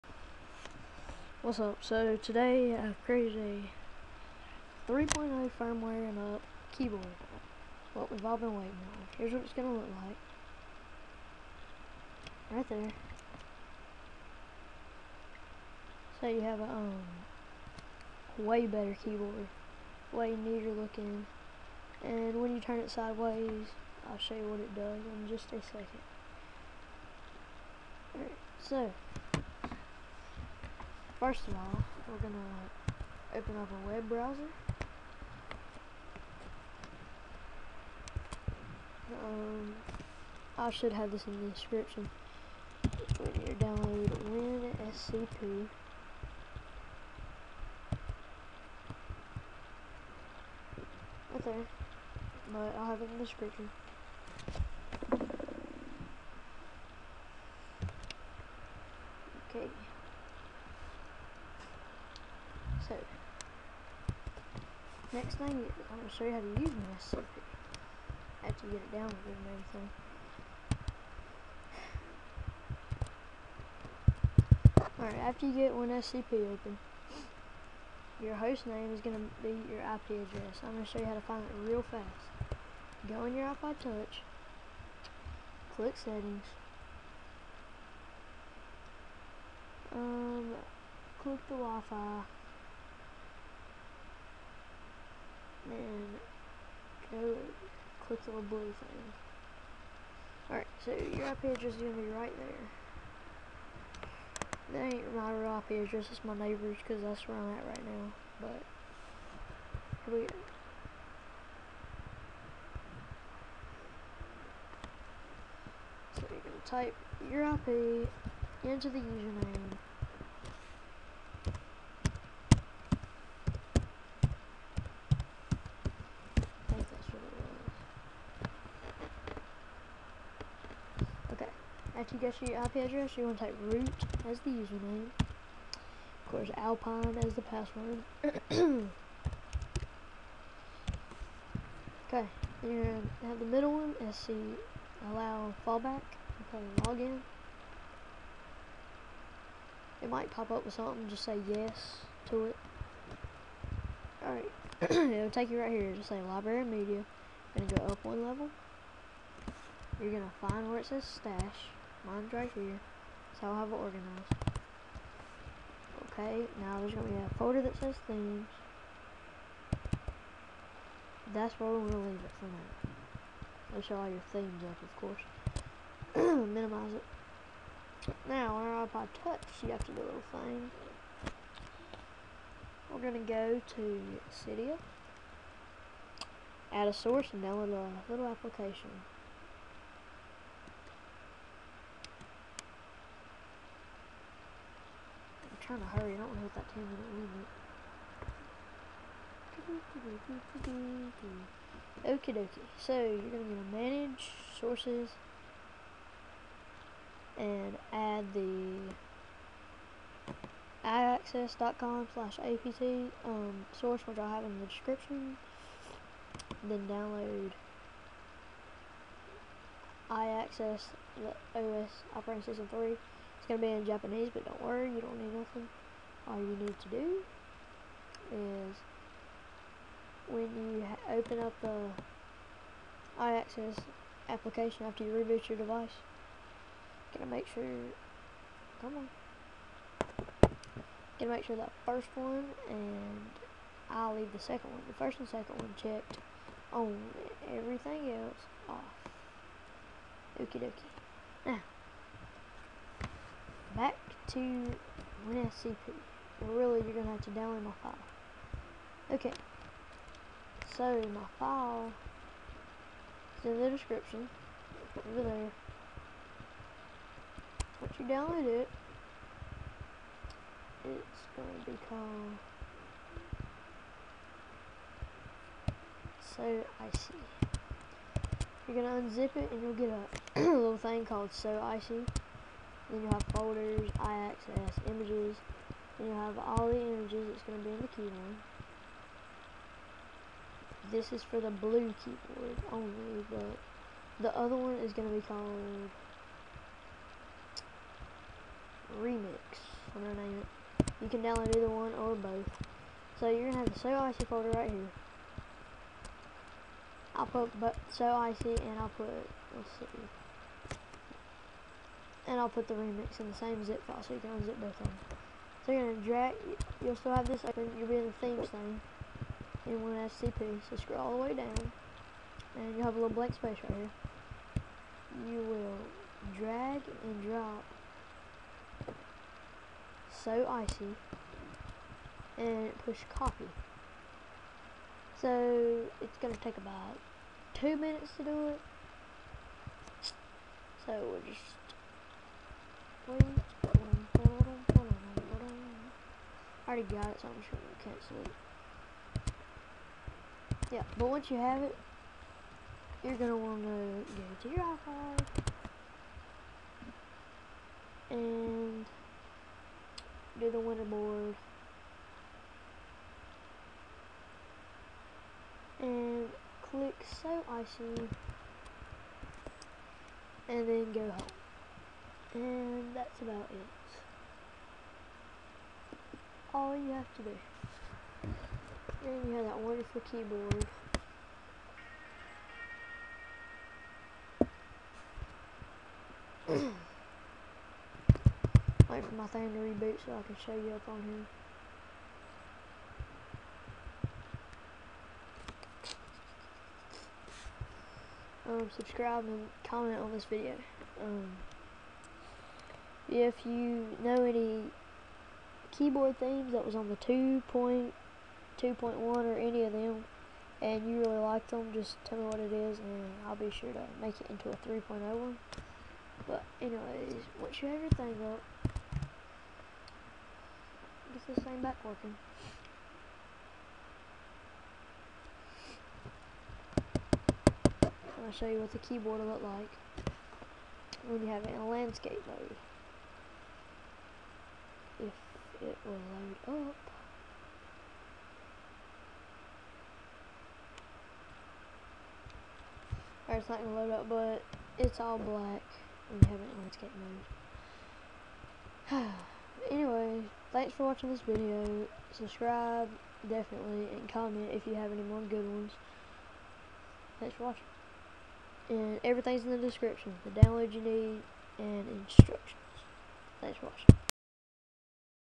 How To Install Keyboards 3.0+ ipod touch iphone
IMPORTANT!!! THE FILE THAT YOU DOWNLOAD WILL HAVE A FOLDER CALLED FOLDERS. PUT THAT IN A FOLDER NAMED WHATEVER YOU WANT THE KEYBOARD TO SHOW UP AS!!!

winscp instructions. go into stash then themes then drop the file into it